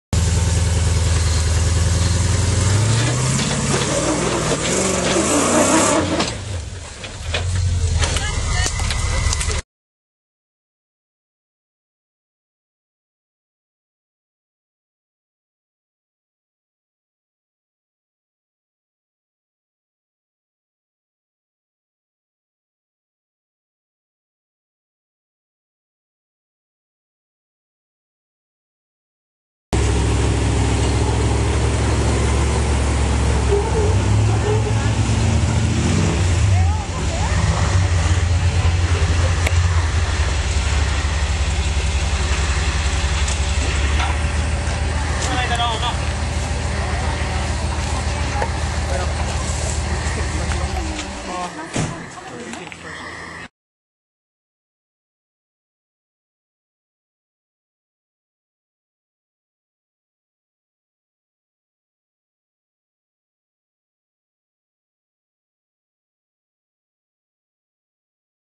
RRCSnap
Range Rover snaps tail shaft while having fun

Oh good its just a tail shaft says owner, better than a crown wheel.